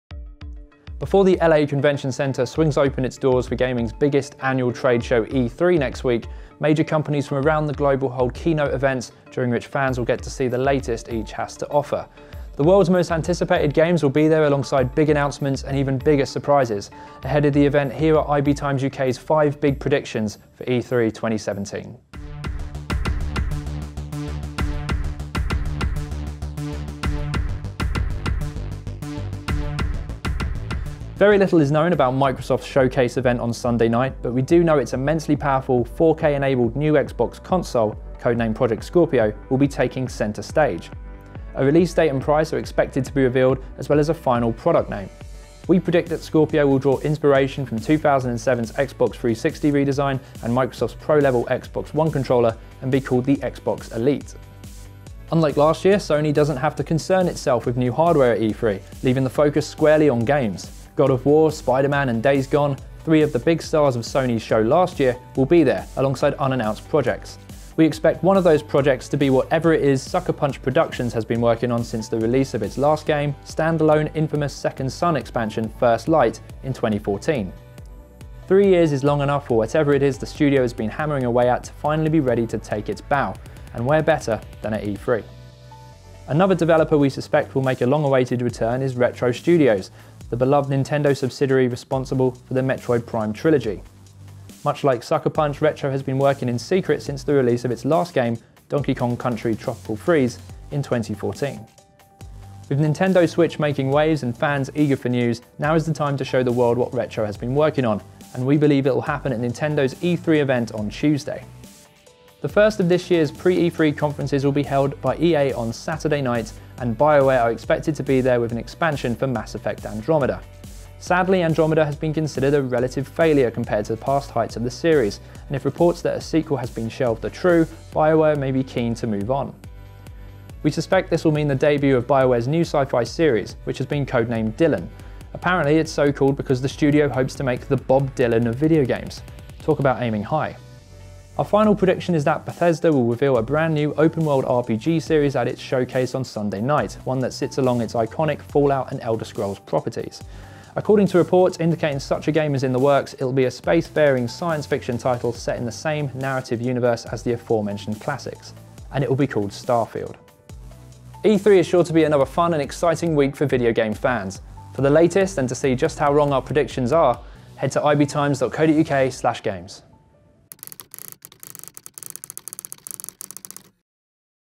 Five big predictions for E3 2017 from Project Scorpio's name to Bethesda's new IP
Gaming's biggest annual trade show E3 kicks off in Los Angeles this weekend with major press conferences from the world's biggest video game companies. IBTimes UK runs down the five big predictions for E3 2017.